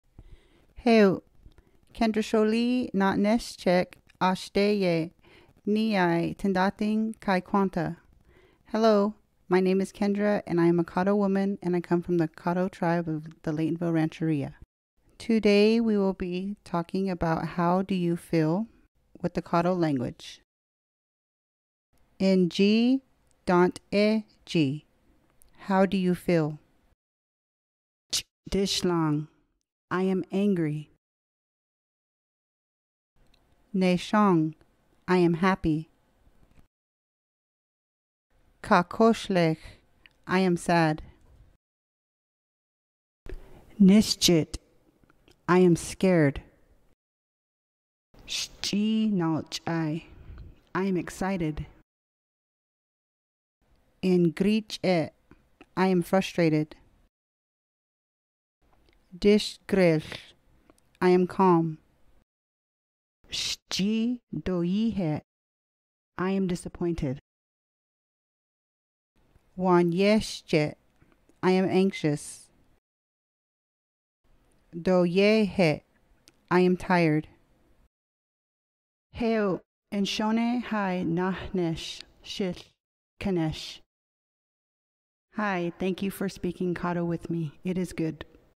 Cahto Feelings
Learn to express feelings in the language of the Cahto Tribe of the Laytonville Rancheria.